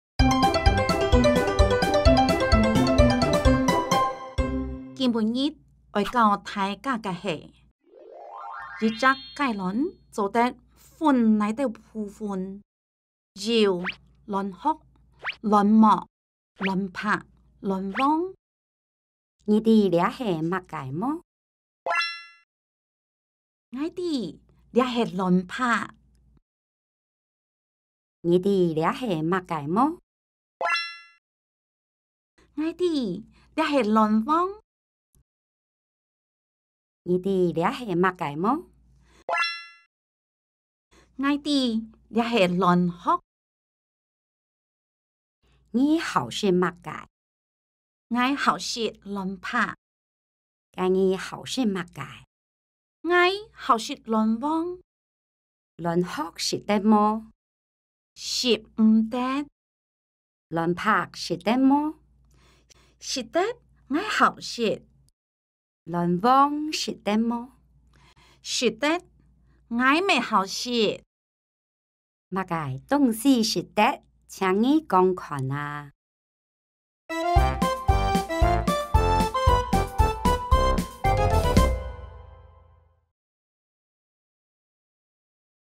【系列課程】臺灣客家語海陸腔｜B01-2一隻雞卵做得分哪兜部分？
今晡日愛教大家个係「一隻雞卵做得分哪兜部分？」

一隻雞卵做得分哪兜部分？
有卵殼、卵膜、卵白、卵黃
A：你知這係麼个無？
B：𠊎知，這係卵白?
B：𠊎知，這係卵黃?
B：𠊎知，這係卵殼?
A：你好食麼个？
B：𠊎好食卵白?
A：該你好食麼个？
B：𠊎好食卵黃?
A：卵殼食得無？
B：食毋得
A：卵白食得無？
B：食得，𠊎好食。
A：卵黃食得無？
B：食得，𠊎乜好食。
A：麼个東西食得？請你講看啊
gimˋ buˋ ngid oiˇ gauˋ tai⁺ gaˋ gaiˇ heˇ 「rhid zhag gaiˋ lonˊ zoˇ ded funˋ nai⁺ deuˋ pu⁺ fun⁺ ？」
rhid zhag gaiˋ lonˊ zoˇ ded funˋ nai⁺ deuˋ pu⁺ fun⁺ ？
rhiuˋ lonˊ hog 、lonˊ mogˋ 、lonˊ pagˋ 、lonˊ vong
A：ngi diˋ liaˊ heˇ mag gaiˇ mo ？
B：ngai diˋ ，liaˊ heˇ lonˊ pagˋ ?
B：ngai diˋ ，liaˊ heˇ lonˊ vong ?
B：ngai diˋ ，liaˊ heˇ lonˊ hog ?
A：ngi hauˇ shidˋ mag gaiˇ ？
B：ngai hauˇ shidˋ lonˊ pagˋ ?
A：gai ngi hauˇ shidˋ mag gaiˇ ？
B：ngai hauˇ shidˋ lonˊ vong ?
A：lonˊ hog shidˋ ded mo ？
B：shidˋ m ded
A：lonˊ pagˋ shidˋ ded mo ？
B：shidˋ ded ，ngai hauˇ shidˋ 。
A：lonˊ vong shidˋ ded mo ？
B：shidˋ ded ，ngai meˇ hauˇ shidˋ 。
A：mag gaiˇ dungˋ siˋ shidˋ ded ？ciangˊ ngi gongˊ konˇ a⁺